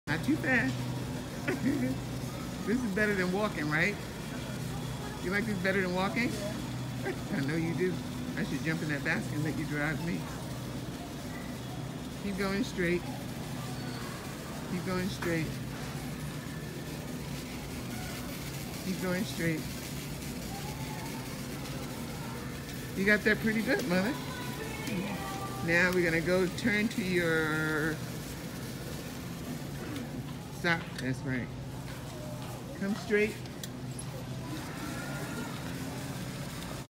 How to Navigate an Elderly/Senior Driving Shopping Cart
How to assist when dementia threatened to derail a life.  Always seek the alternative.